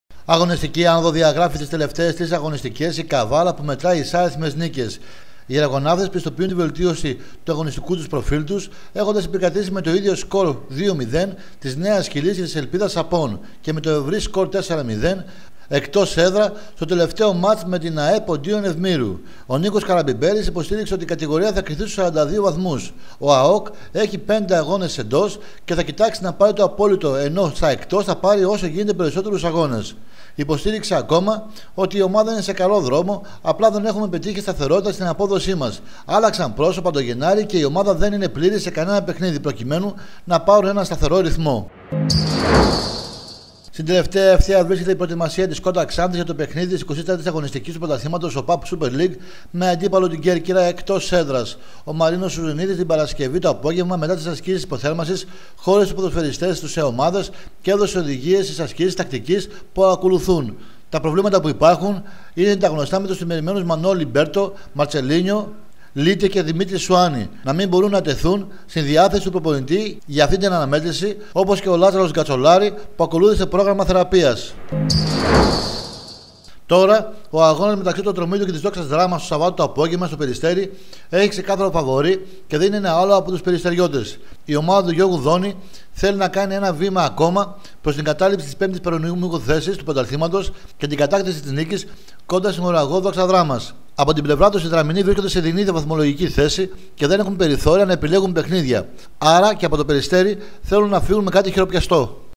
ΑΘΛΗΤΙΚΗ ΚΙΝΗΣΗ, 10/3/2012
Αγωνιστική άνοδο διαγράφει τις τελευταίες τρεις αγωνιστικές η Καβάλα που μετράει ισάριθμες νίκες. Στην Ξάνθη Στην τελική ευθεία βρίσκεται η προετοιμασία της SKODA ΞΑΝΘΗ για το παιχνίδι της 24ης αγωνιστικής του Πρωταθλήματος ΟΠΑΠ Superleague με αντίπαλο την Κέρκυρα εκτός έδρας. Και στην δραμα στο εκτος εδρας παιχνιδι οι Δραμινοί βρίσκονται σε δεινή βαθμολογική θέση και δεν έχουν περιθώρια να επιλέγουν  παιχνίδια. Αρα και από  το Περιστέρι θέλουν να φύγουν με κάτι χειροπιαστό.